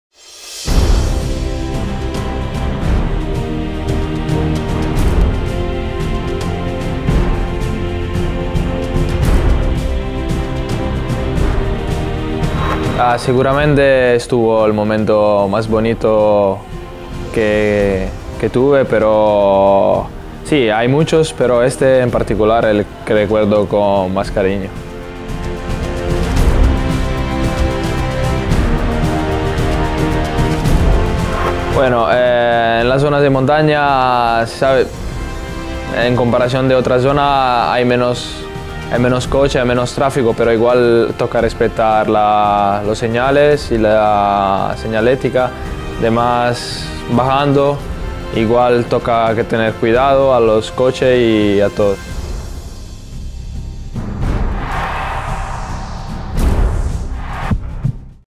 Danilo Celano, el escalador italiano del Caja Rural-Seguros RGA
Danilo Celano fichó por el equipo Caja Rural-Seguros RGA en la segunda parte de la temporada 2017. Había sido protagonista en el montañoso Giro de los Apeninos, donde consiguió la victoria. Sueña con correr una de las tres grandes vueltas.